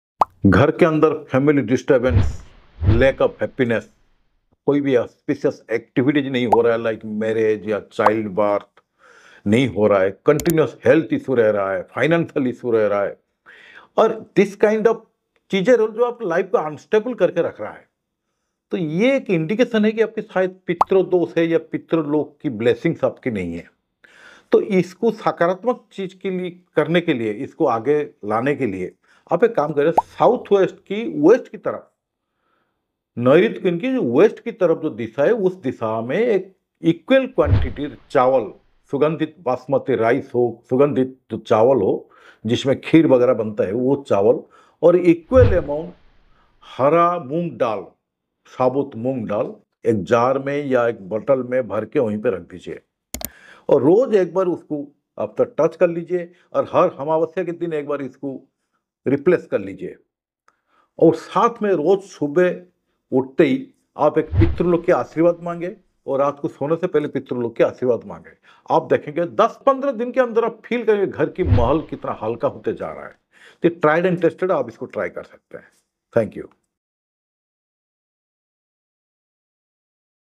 Pitrudosh Problems and Remedies
Due to PITRUDOSH You are Facing Family Disturbance, Lack of Happiness, No marriage, No Childbirth, Health & Financial Issues Follow these Steps for Remedy.

Consult For:
✅ Vastu
✅ Maha Vastu
✅ Numerology
✅ Healing
✅ Astro Vastu Visiting Card
✅ Scientific Logo Design